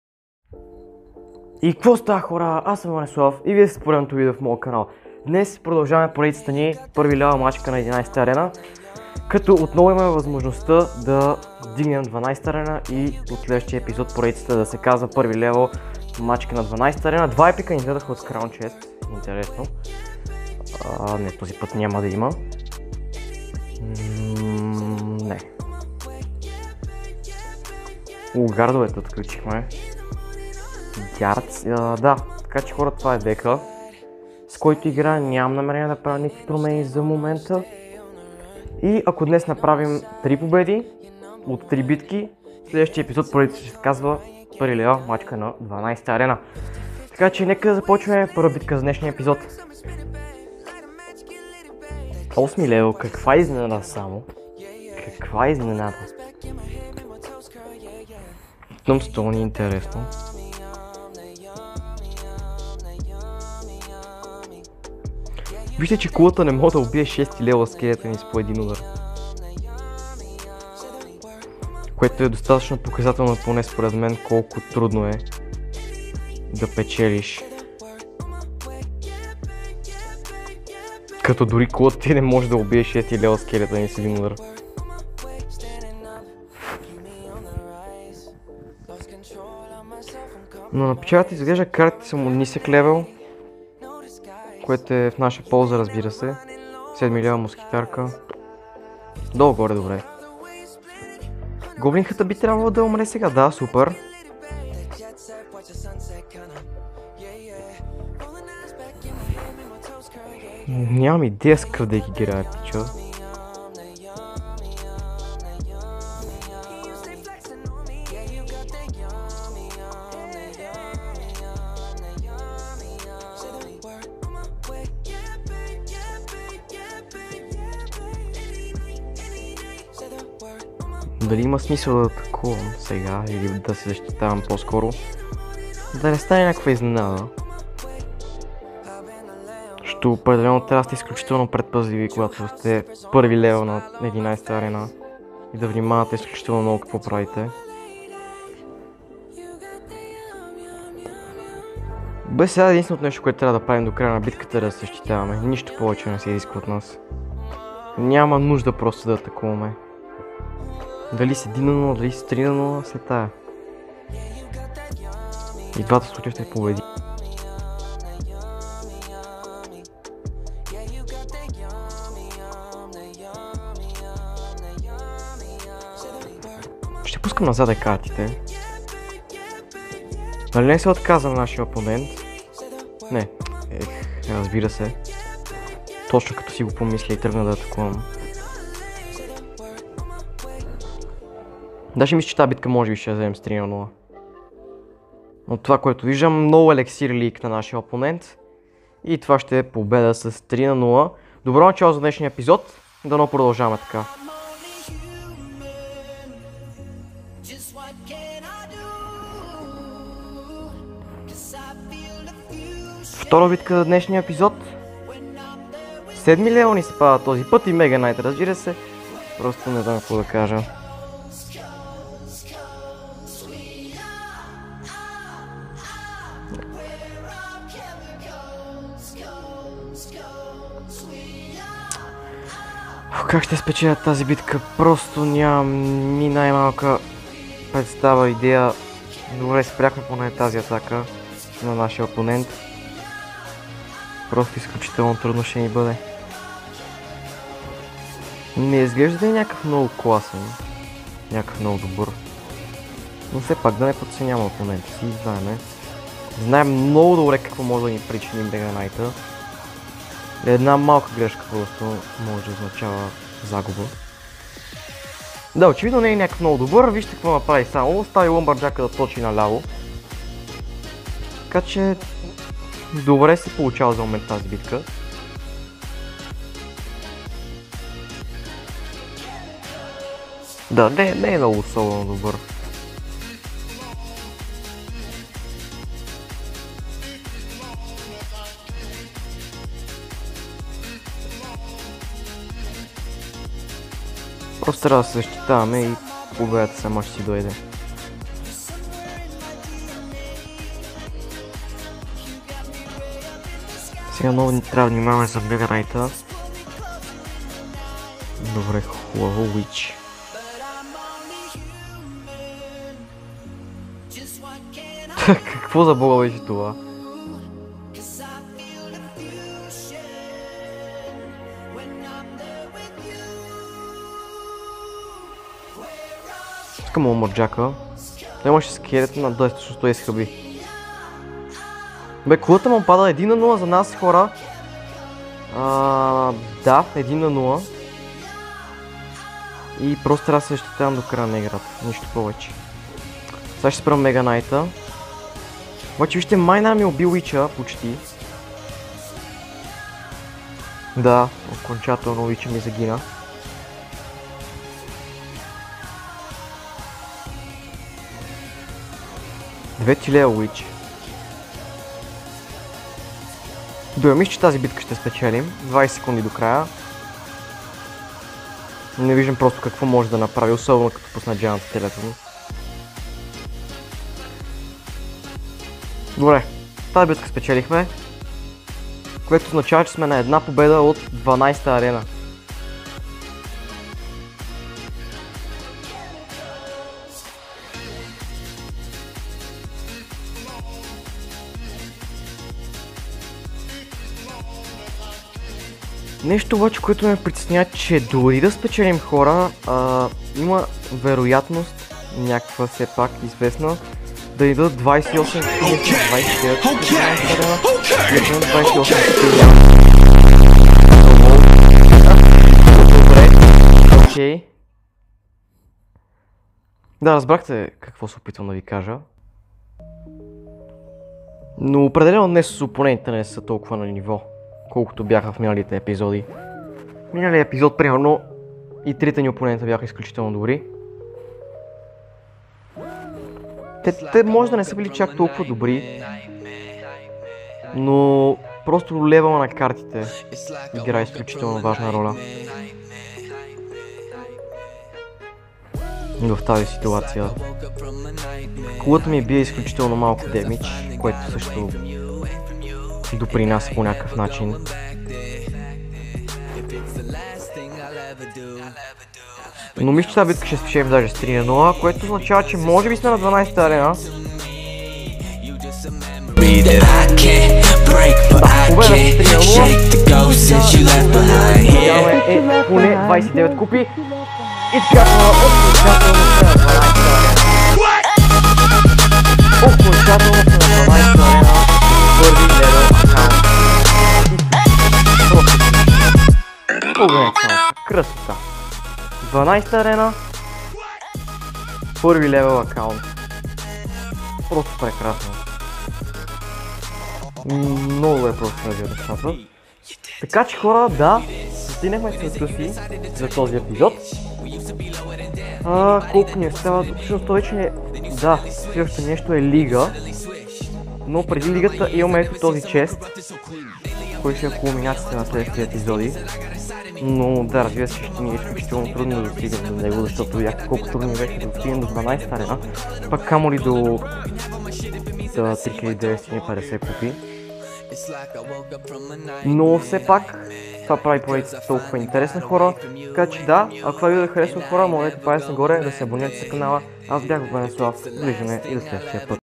Вдигаме 12-та арена с 1-ви левел акаунт?! Първи левел мачка на 11-та арена #4
Можете да се свържете с мен